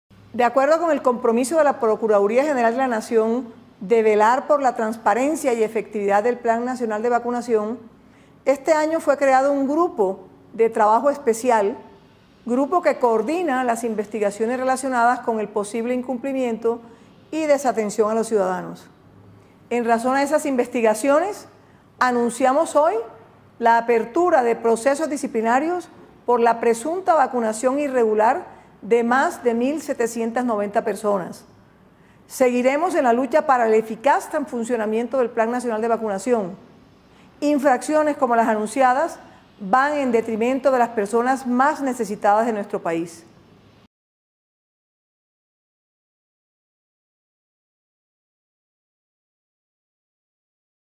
La PGN adelanta procesos disciplinarios por presunta vacunación irregular de más de 1.790 personas
Bogotá 14 abril de 2021

- Investigaciones disciplinarias vinculan a Director Subred Norte (Bogotá) y alcaldesa de Icononzo (Tolima) por la aplicación de la primera dosis a 441 ciudadanos, que al parecer, no cumplían los requisitos.

- En los departamentos de Norte de Santander, Valle del Cauca, Chocó, Cundinamarca, Córdoba y Caquetá se adelantan diez indagaciones preliminares.